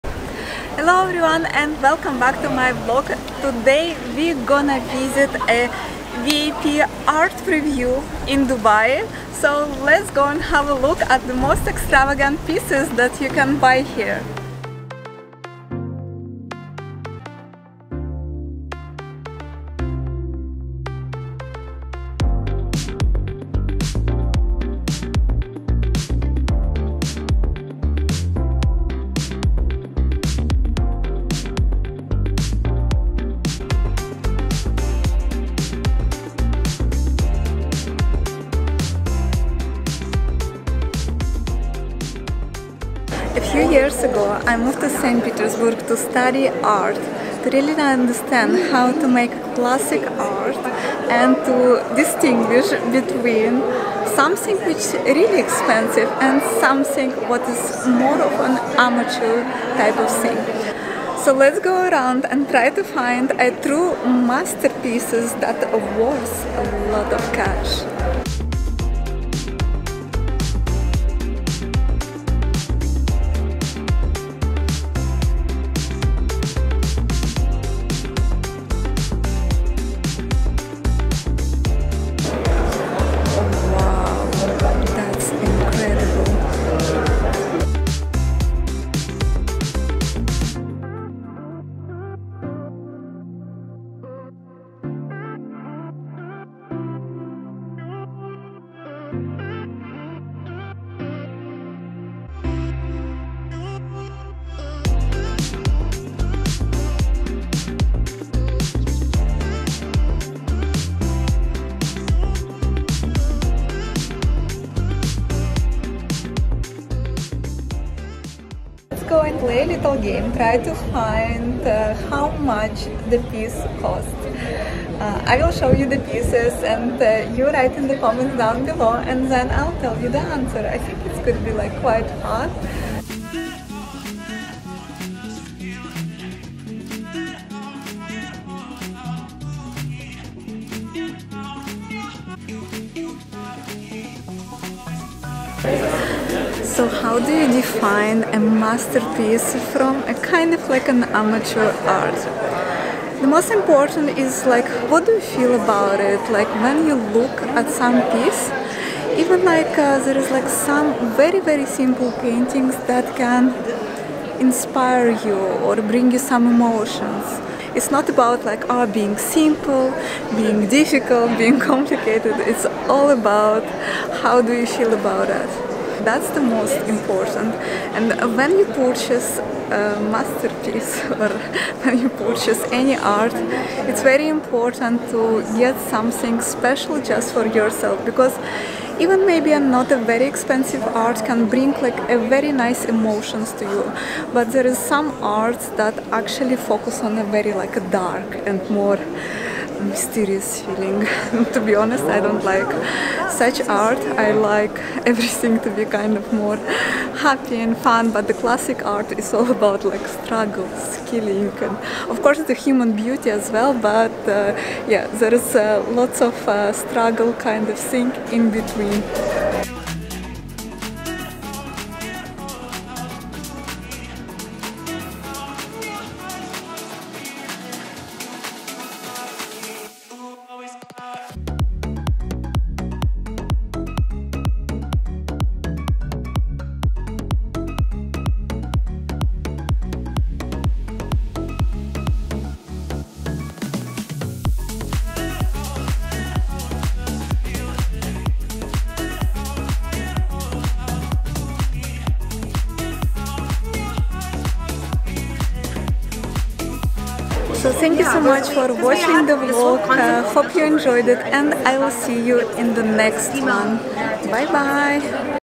Hunting for Expensive Art Masterpieces in Dubai| VLOG 16
In this episode, I am taking you with me to VIP preview of Art Dubai 2024 exhibition. If you are living here, it is yearly event not to miss.